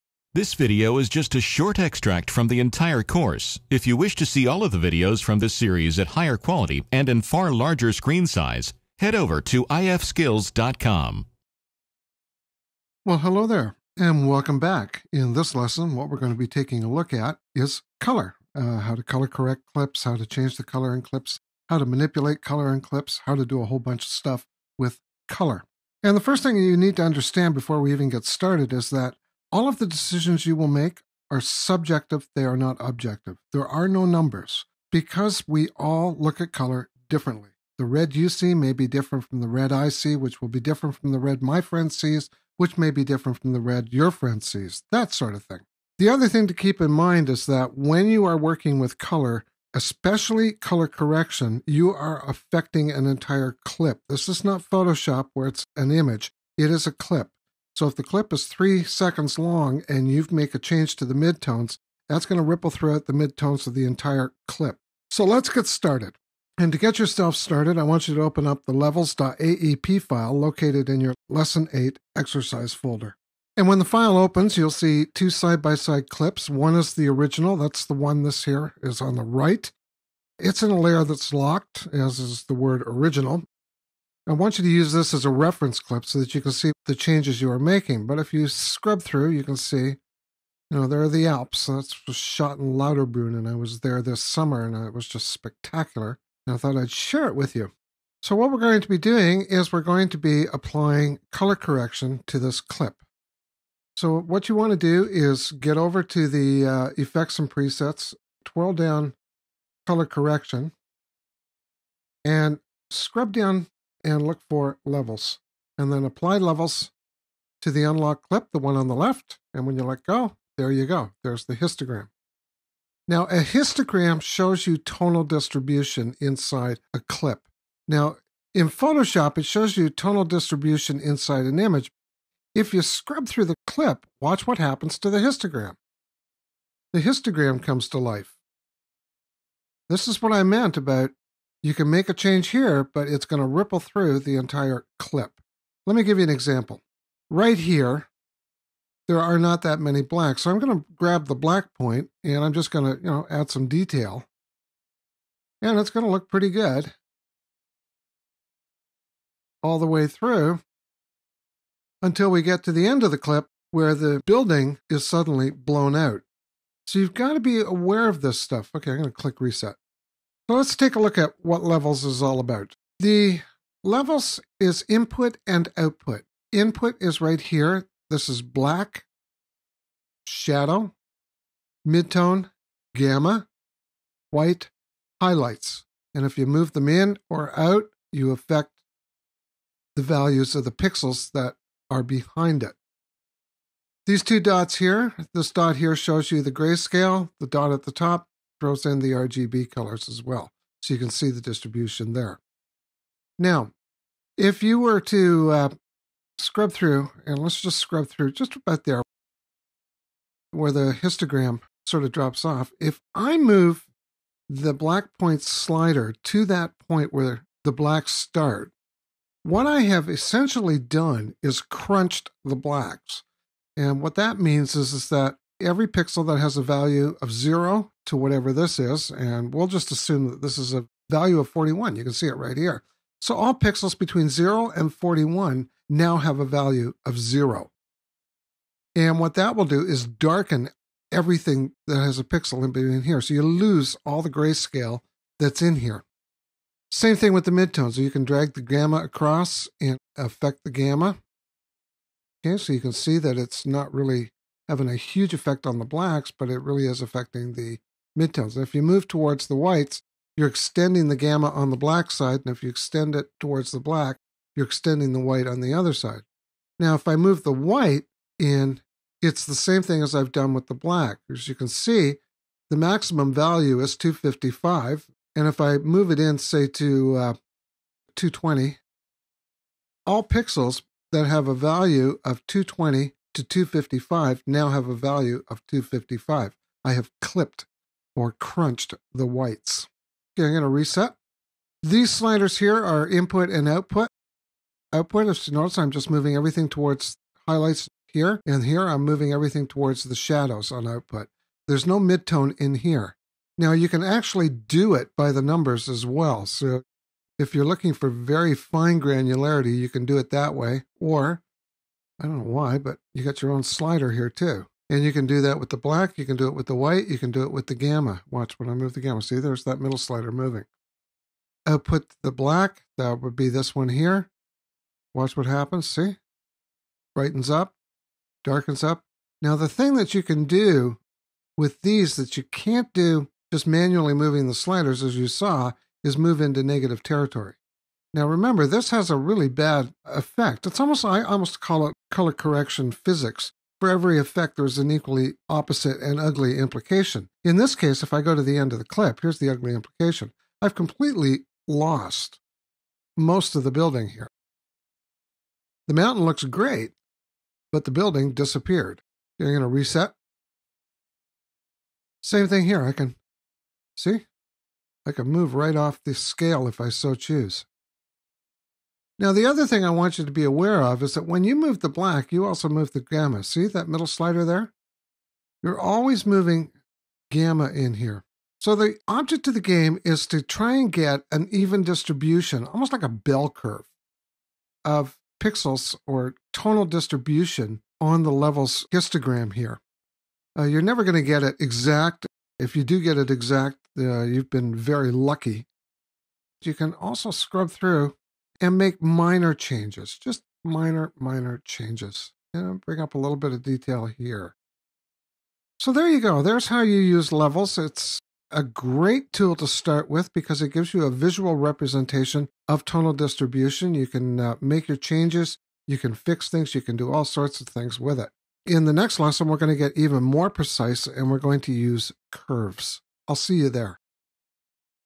After Effects CS5.5 Adjustment Levels Made Easy.
Want all of our free After Effects videos?

In this video based course expert author Tom Green shows you the techniques to take your Adobe After Effects skills to the next level. This tutorial video covers Adjustment Levels in Video. This lesson is just a short extract from the full 11 hour course.